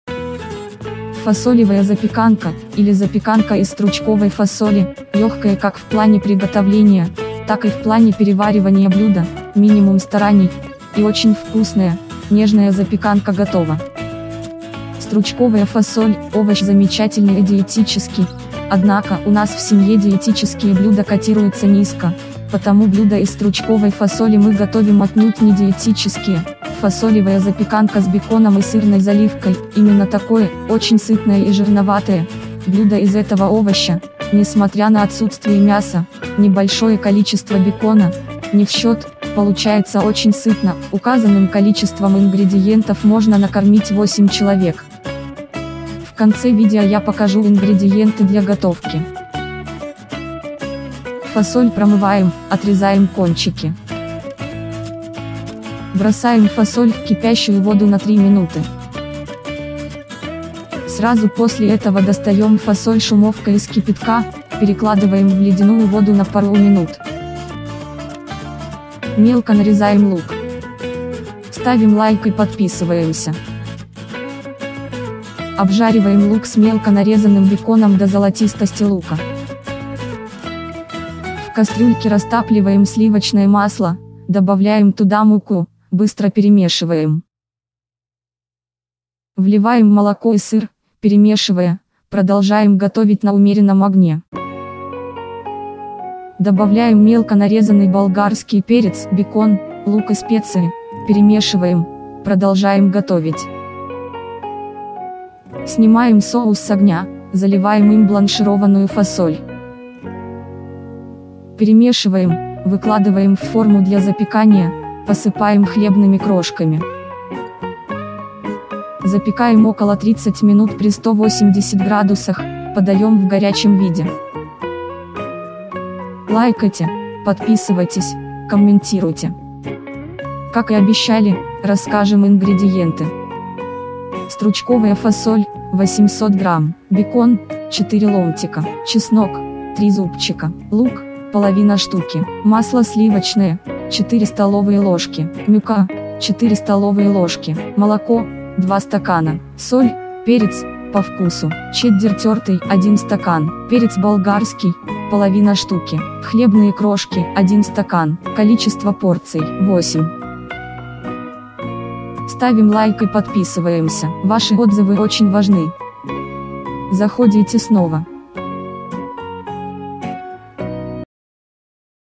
★БЛЮДО ПРЕЗИДЕНТА.★ ФАСОЛЕВАЯ ЗАПЕКАНКА ★
★Блюдо мистически вкусное● Фасолевая запеканка, или запеканка из стручковой фасоли - легкое как в плане приготовления, так и в плане переваривания блюдо● Минимум стараний - и очень вкусная, нежная запеканка готова●